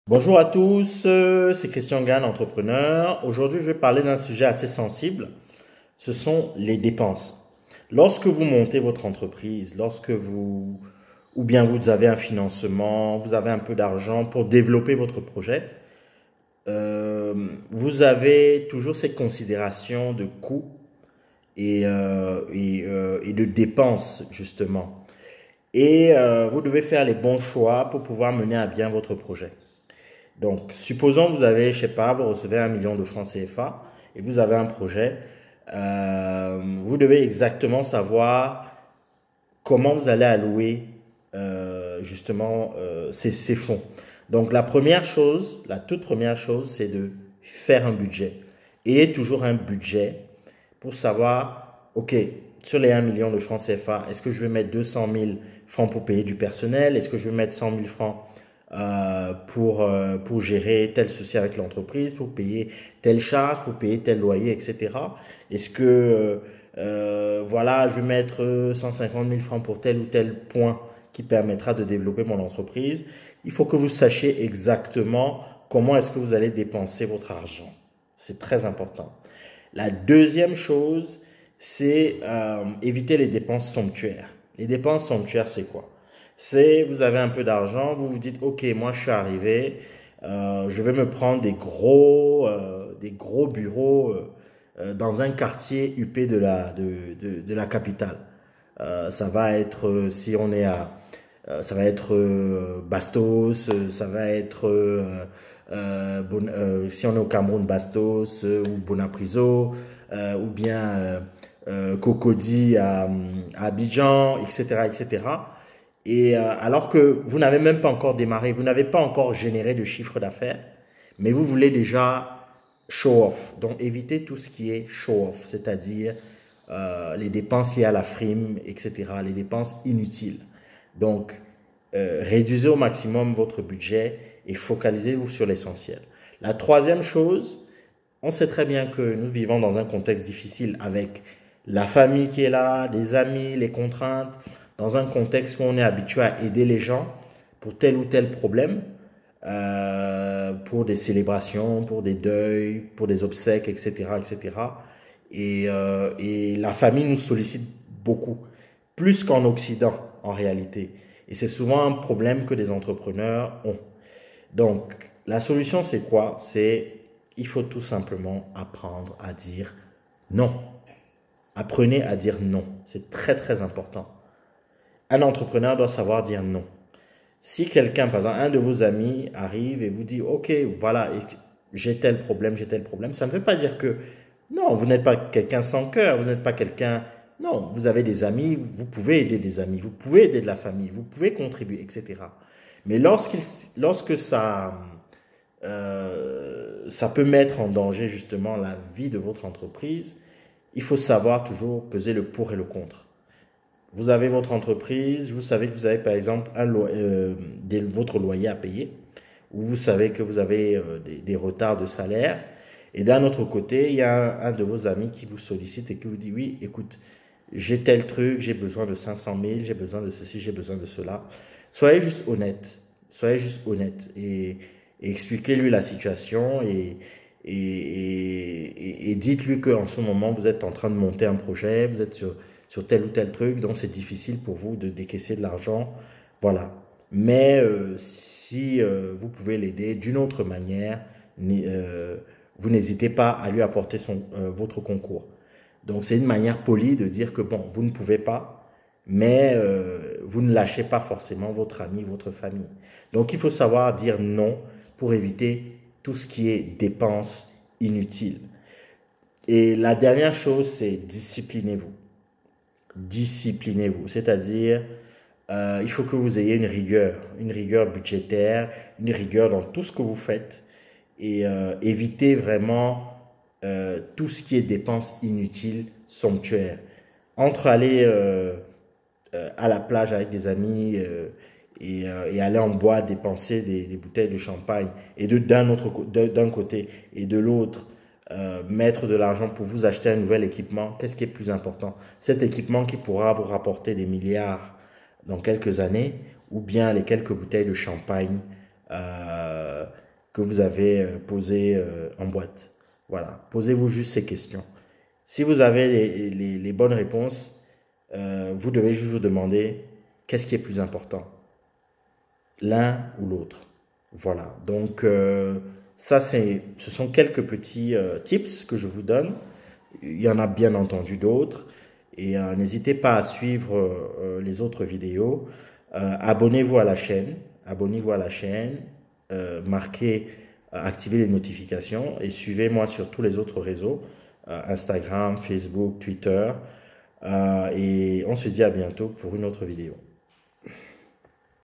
Comment éliminer les dépenses inutiles ?
Vous démarrez un projet, vous vous battez, vous cravachez...mais vous vous rendez compte que çà n'avance pas ? Ou va l'argent ?

Christian Ngan  (Entreprenariat)

Madlyn Cazalis (Industrie)